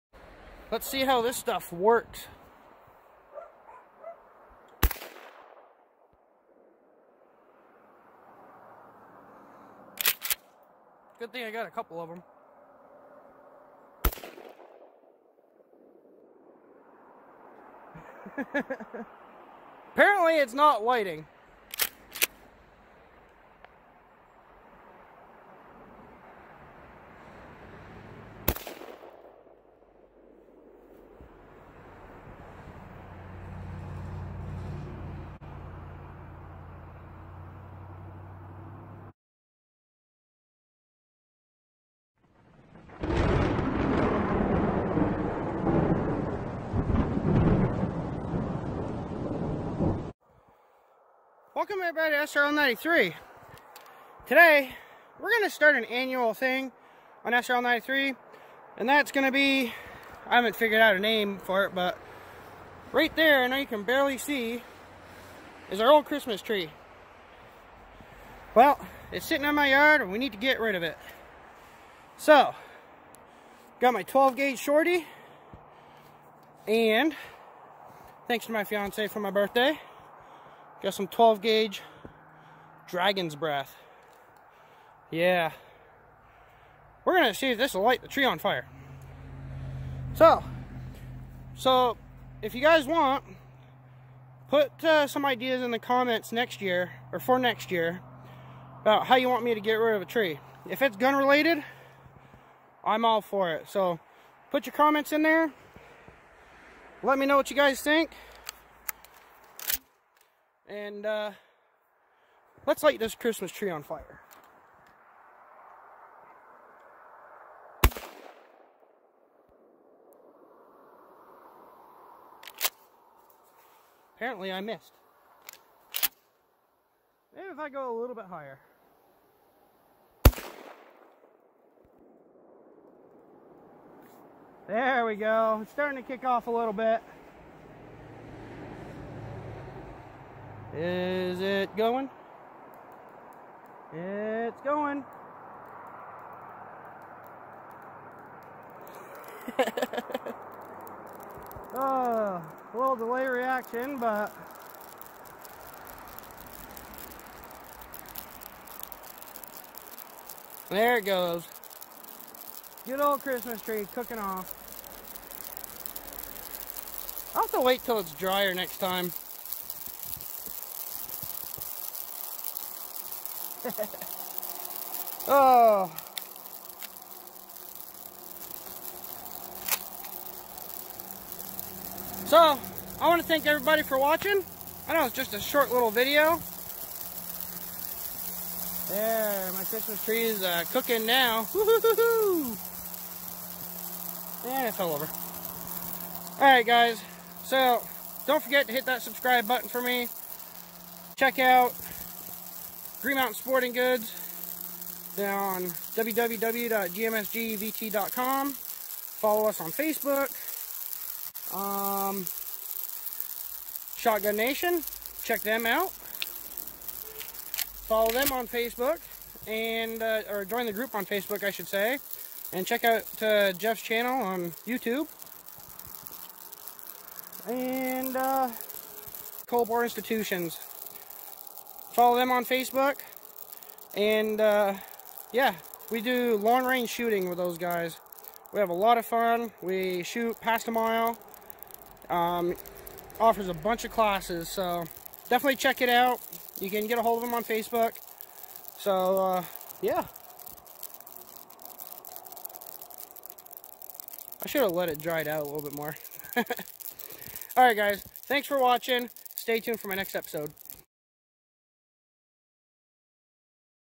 1st Annual X-Mas Tree Burning
Starting this year SRL 93 is going to do an annual video on burning the Christmas tree after the holidays. Thanks to my lovely fiancé, she had gotten me Dragons Breath for my Birthday. Lets see how this works lighting a Christmas tree on fire.